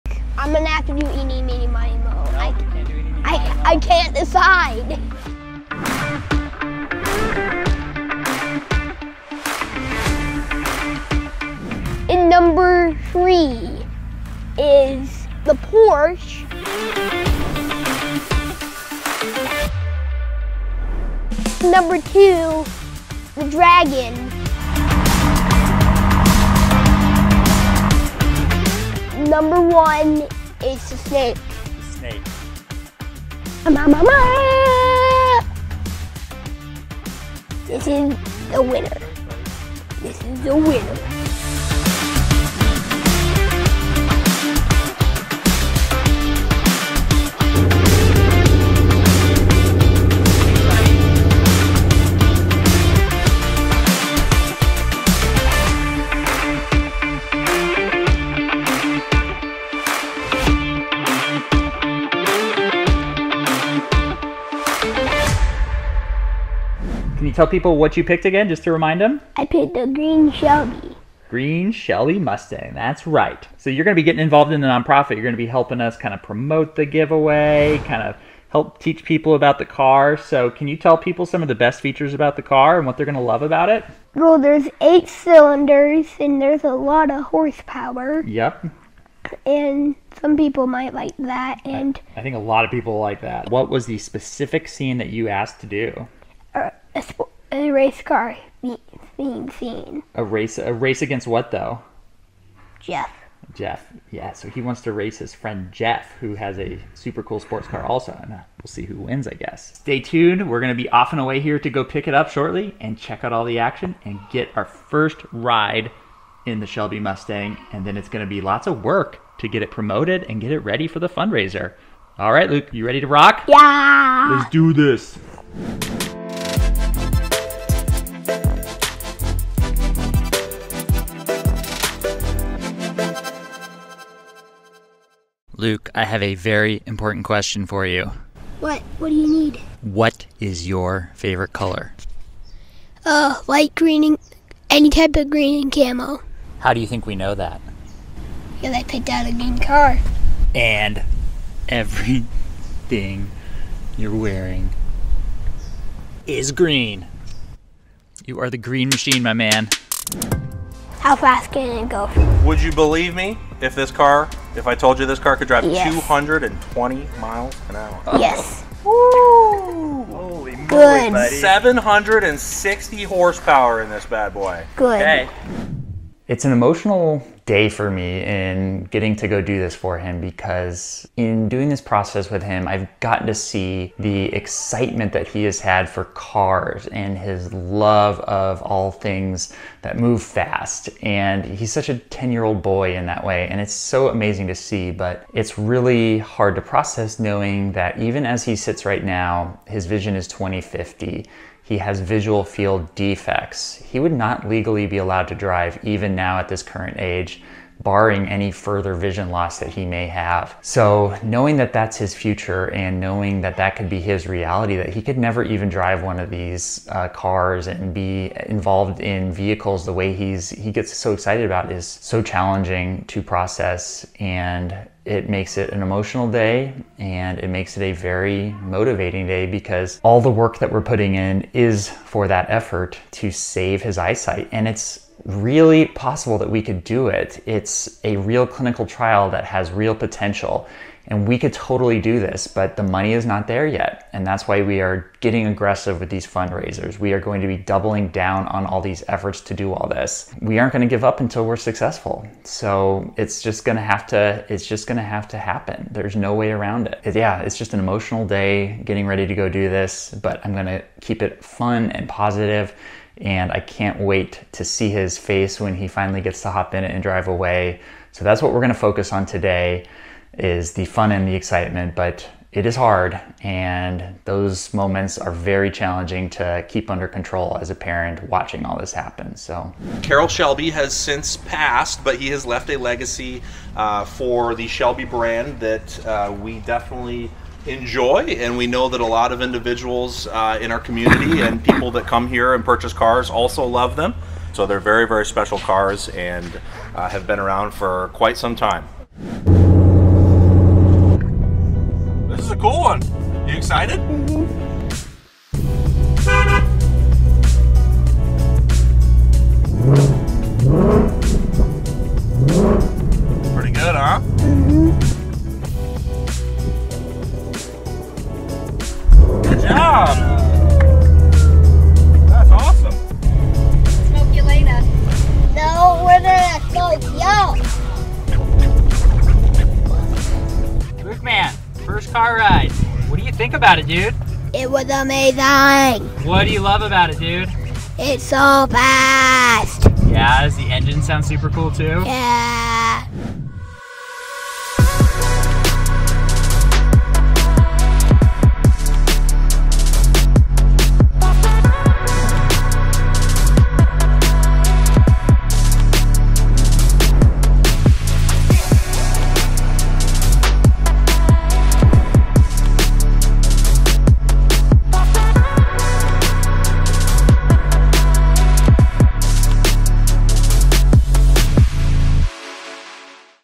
You can WIN this $100,000 Shelby GT500 that LUKE picked out for our current fundraiser!
You can win a $100,000 Shelby GT500 - the FASTEST factory Ford ever made - PLUS $30,000 cash in our nonprofit giveaway fundraiser.  The best part is, LUKE picked this vehicle.  Luke LOVES all things that go fast - as 10 year old boys tend to - and it was an amazing moment for him getting to pick this vehicle.  Check out the excitement when we picked it up for the first time from our friends at Lawrence Motorsport in Scottsdale, AZ.

We founded our nonprofit because our son is going blind due to a rare genetic condition called retinitis pigmentosa caused by Bardet-Biedl Syndrome.  We are on the cusp of a cure that could save his eyesight and thousands of children like him - BUT - we do not have the necessary funding yet to make the clinical trial a reality.

That is where YOU come in.  Funding for rare disease research is done by people like YOU in fundraisers like THIS ONE.  Your entry to win this Shelby GT500 will directly support that clinical trial.  This trial could become a reality in 2024 with your support.  Please head over to our website to make your entry and have a chance to win this amazing vehicle.